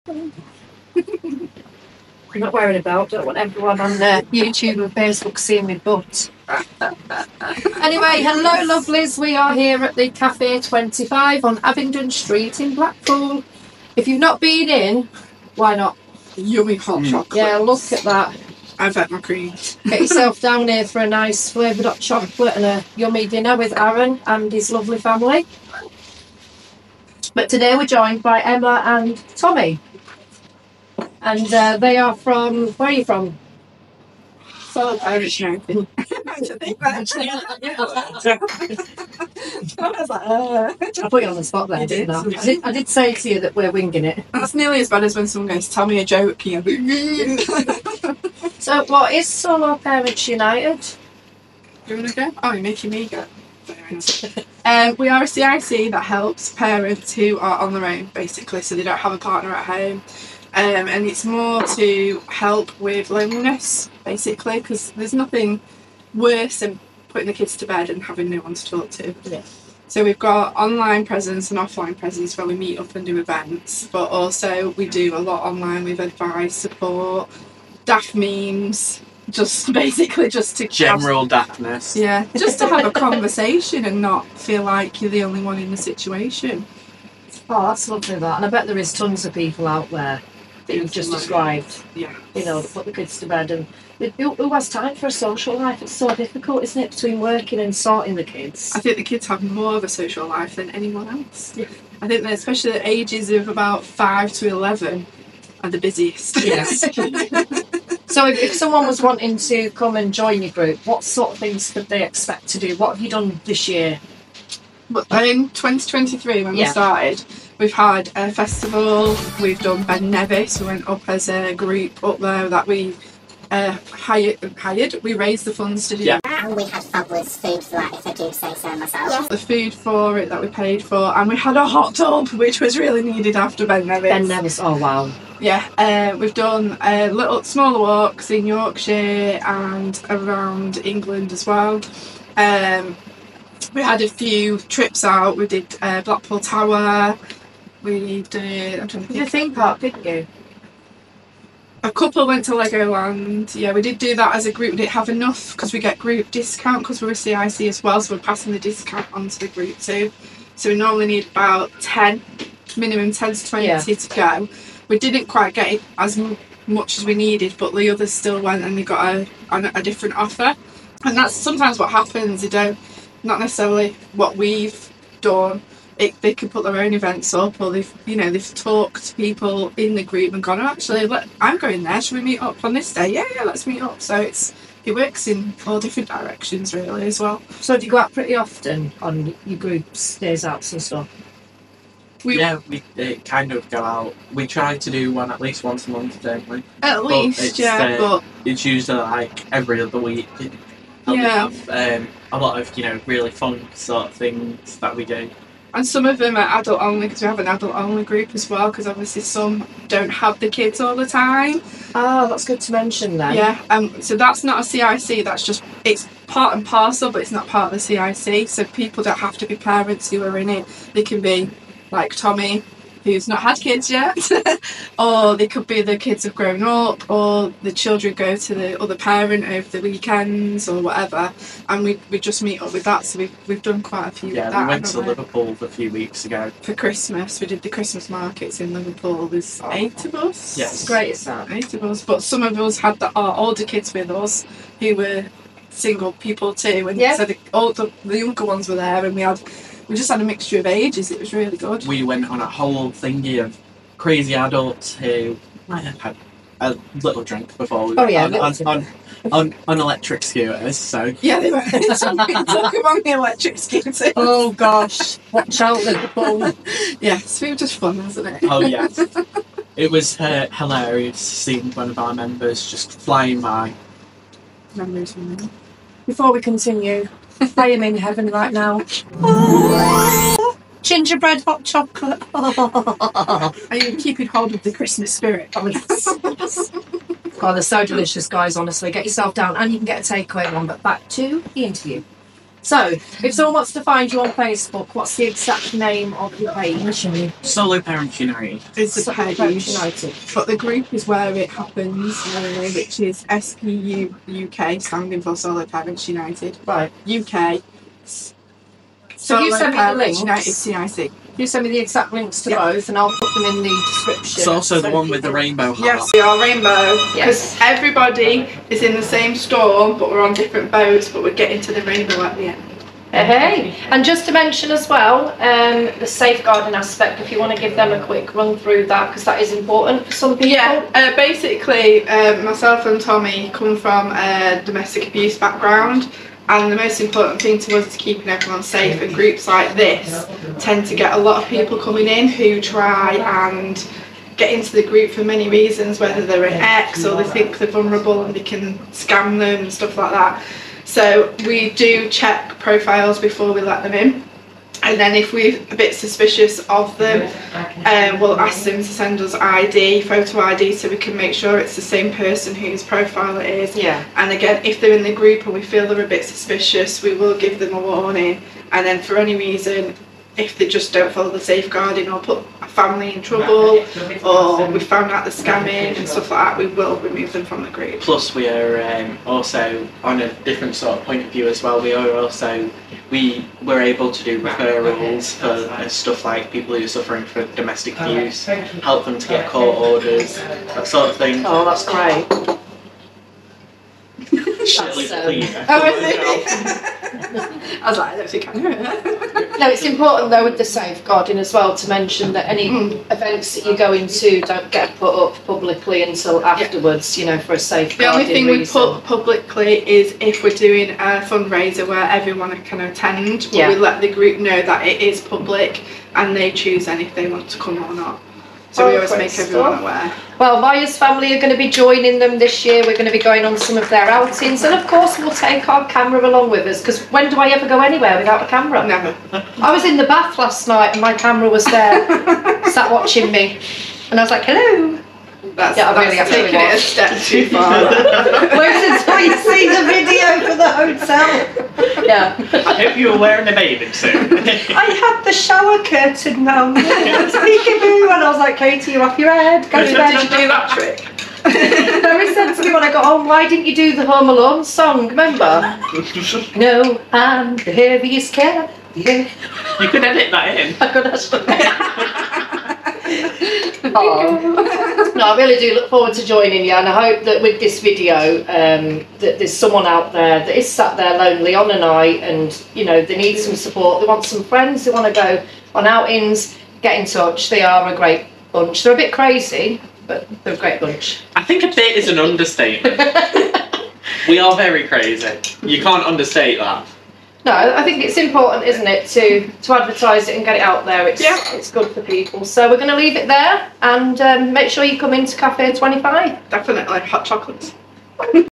Blackpool Solo Parents United, what is it??
Who are Solo Parents United here we meet up at Cafe 25 Blackpool  and find out about solo parents united 😁

They  are a UK wide group who help, support and arrange meetups and events so no solo/single parent has to feel lonely or on their own struggling.

They have helped financially, helped some over anxiety to meet up and make friends. Put on an amazing summer festival for all families to attend and enjoy.  Plus got 7 parents up Ben Nevis and on a weekend away. Lovely Christmas party with the Grinch. Had a great camping trip and more to come in 2024.

Page

Lots more to look forward to.

The aim is to combat loneliness and encourage friendships.

We might not all be on the same boat but we are definitely fighting in the same storm 🌈

"If your a solo/single parent come join our groups if not please support anyway you can we put a few public fundraising events on throughout the year and always looking for anyone or companies that can help us.

they  only have one group for parents as its uk wide and all can go on all events if you want to and chat online.

what a lovely group I bet it's a lifeline to some people